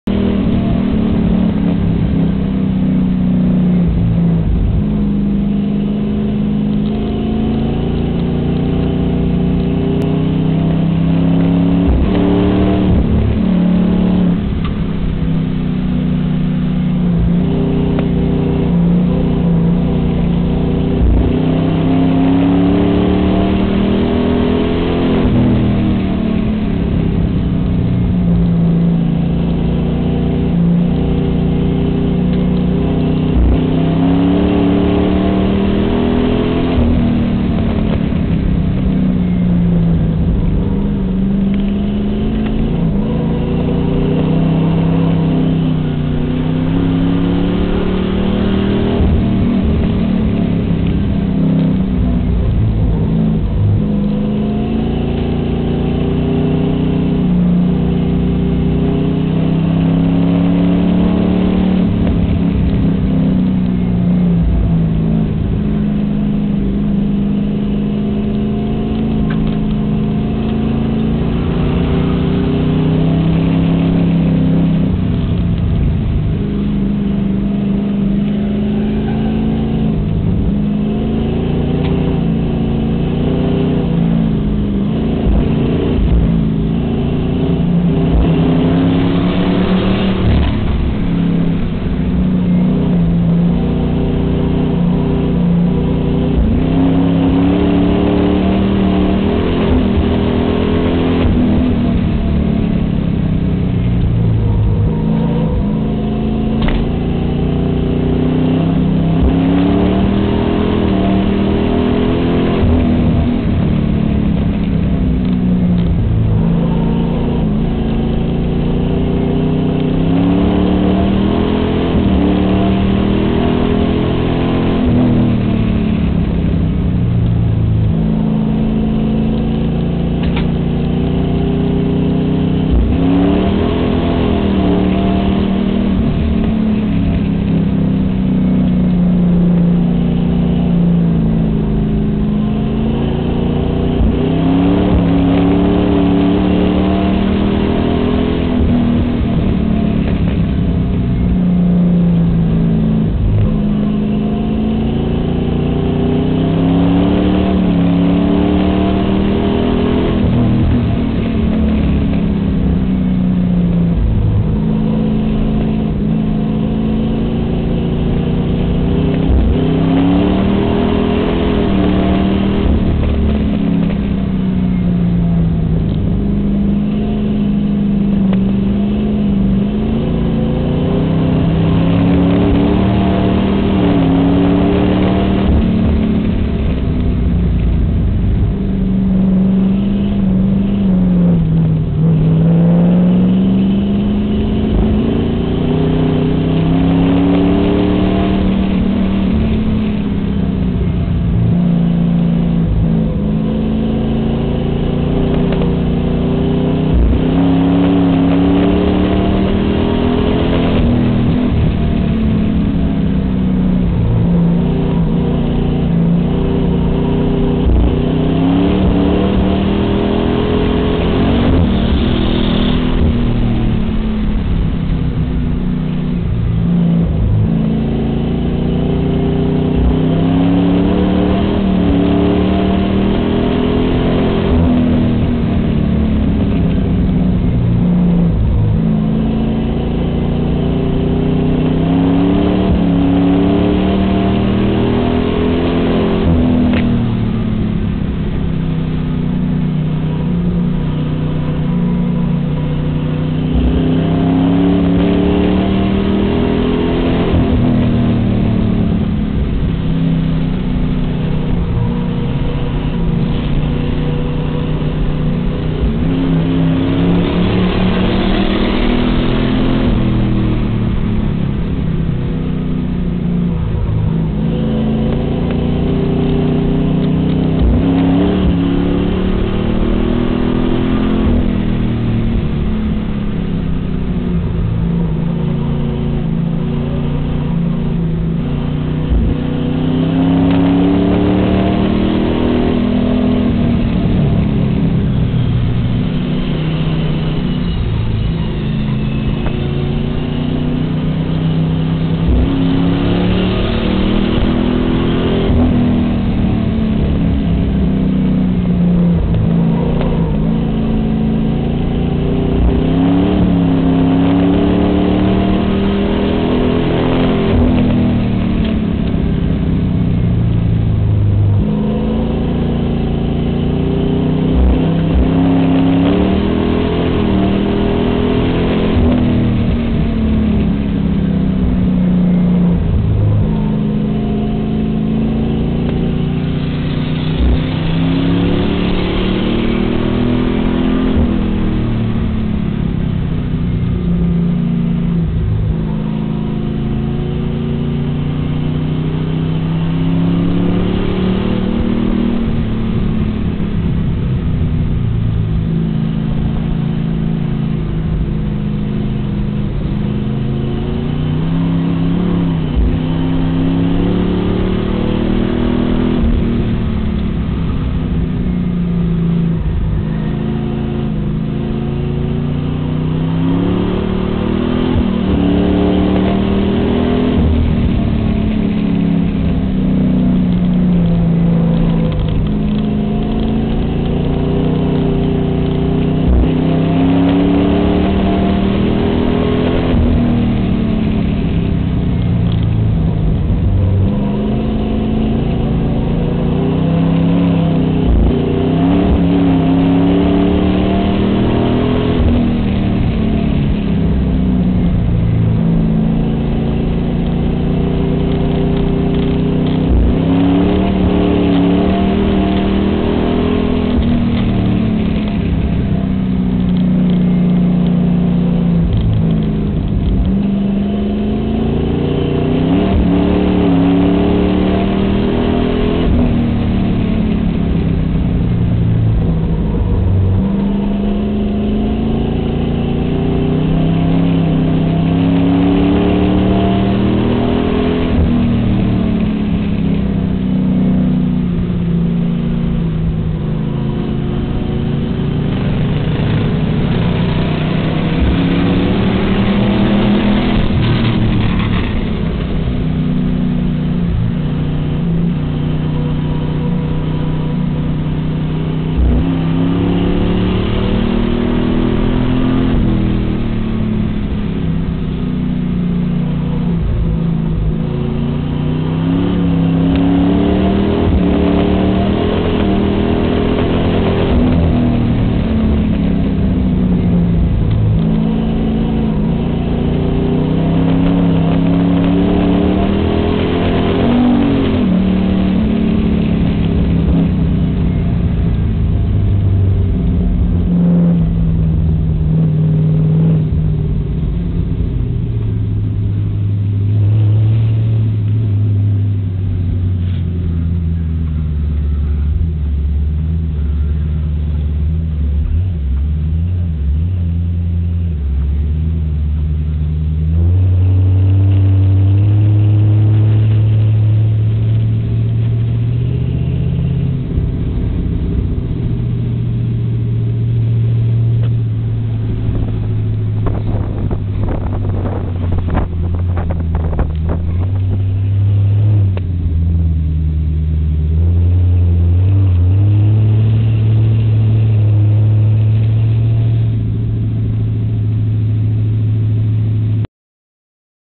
(5of5)Shasta Raceway Park 200 Lap Enduro 3/20/11
Shasta Raceway Park 200 Lap Enduro 3/20/11.

1st) David Papst Jr, 2nd) Wess Brown, 3rd) Chris Lancaster, 4th) Ron Wills